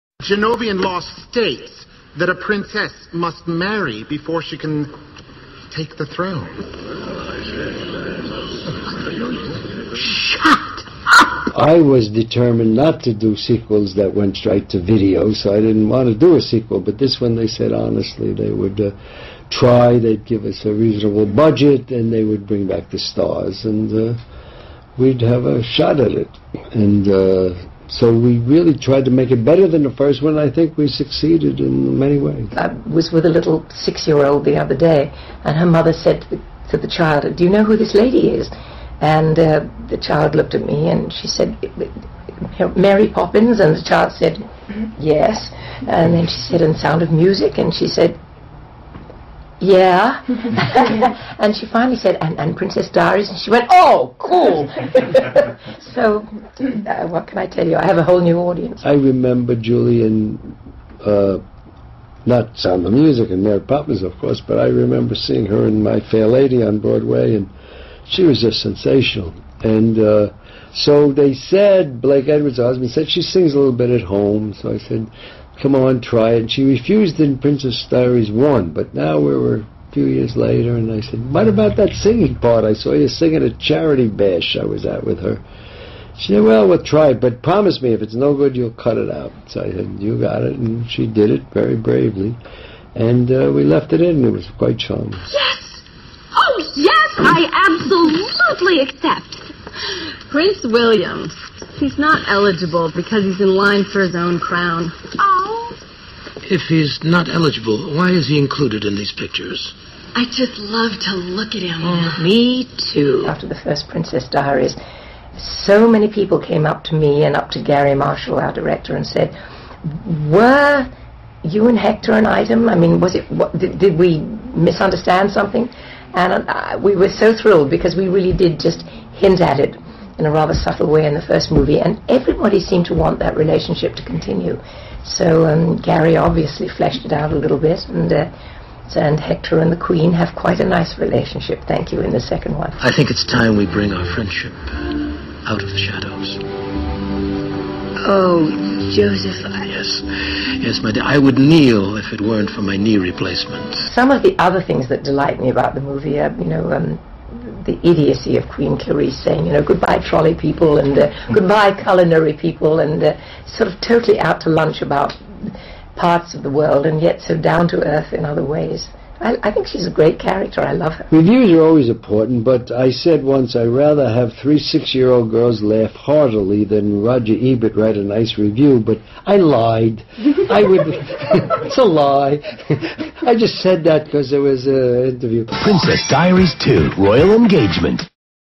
(Short) Princess Diaries 2 Interview with Julie Andrews and Garry Marshall (2004)
Talking about a how the new generation doesn't recognize her for Mary Poppins or TSOM, but for her role in Princess Diaries.
Garry Marshall mentions that he was tipped off by Blake Edwards that Julie sings at home again after the disasterous medical procedure that took her singing voice.